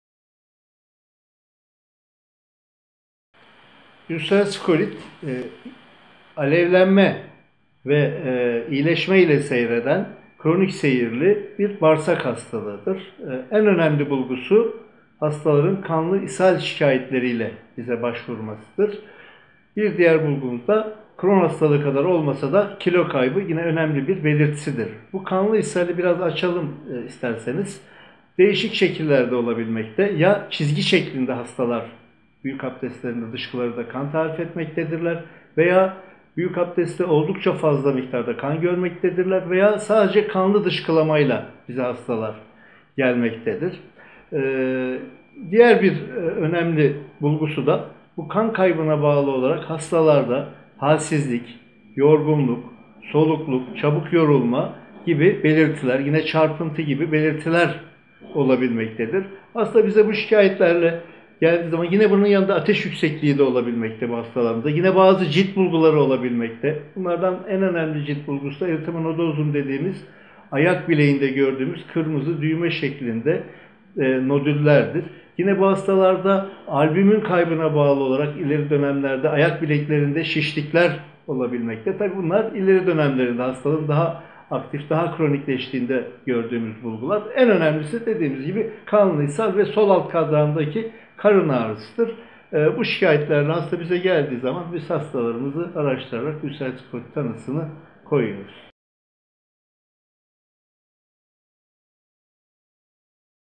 Ülseratif kolit nedir? Belirtileri nelerdir?
Ülseratif kolit nedir? Belirtileri nelerdir? diye sorduk. Türkiye Yüksek İhtisas Hastanesi'nden Prof.Dr.Ertuğrul Kayaçetin cevapladı.
İBH ile ilgili diğer sorularınız için sitesini ziyaret edebilirsiniz.